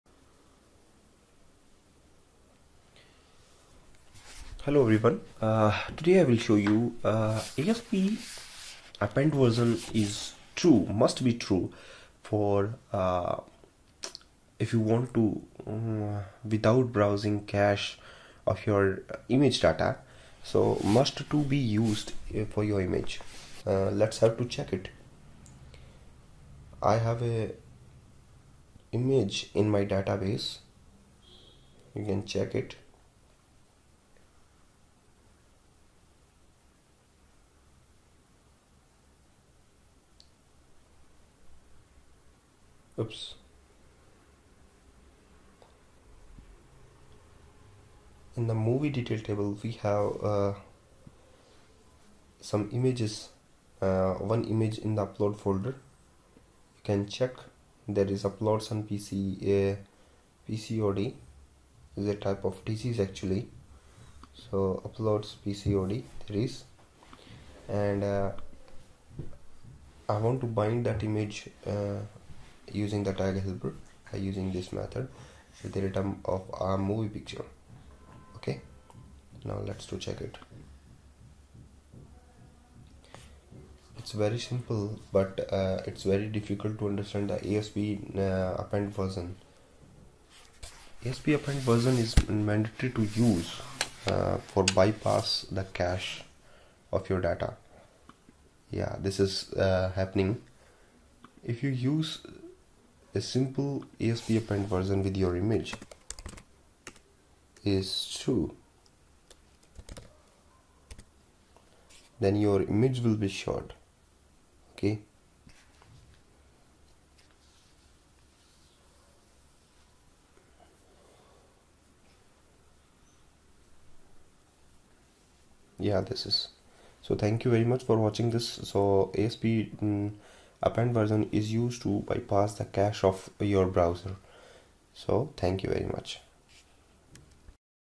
asp-append-version should true for Image in ASP.NET CORE | ASP.MVC 6
Images are not displaying after pass the correct data because you are not using asp-append-version =true attribute in the image tag.

How does javascript version (asp-append-version) work in ASP.NET Core MVC

Should I be appending asp-append-version=“true” to all my images in asp.net MVC6?